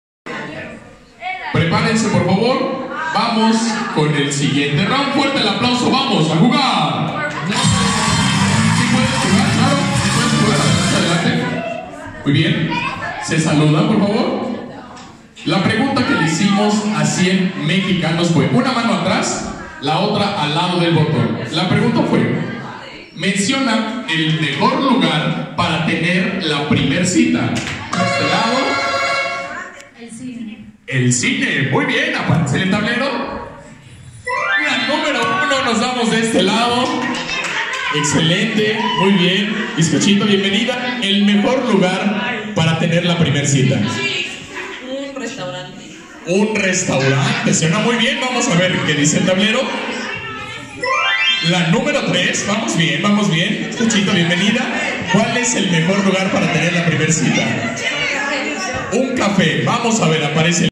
CIEN MEXICANOS DIJERON CONDUCIDO POR EL IMITADOR DEL VITOR EVENTO EN PUEBLA. WHATS 551353-8159(3)
CIEN MEXICANOS DIJERON CONDUCIDO POR EL IMITADOR DEL VITOR EVENTO EN PUEBLA. WHATS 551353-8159 TEL.